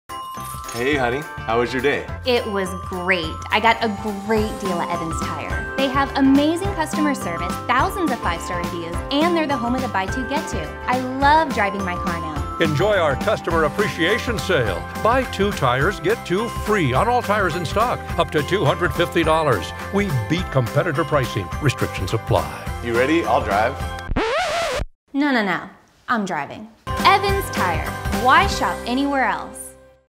Evans Tire & Service Centers - Customer Appreciation Sale Buy 2 Tires, Get 2 Free
We are having a customer appreciation sale! Come on in and enjoy our Buy 2 Get 2 at Evans Tire and Service Centers!

Buy 2 tires, Get 2 FREE on ALL tires in stock!! Up to $250!
Restrictions apply. See store for details.
Offer ends 8/18/19.

We beat competitor pricing!!!

Hurry into your nearest Evans Tire & Service Center today - with 18 convenient San Diego locations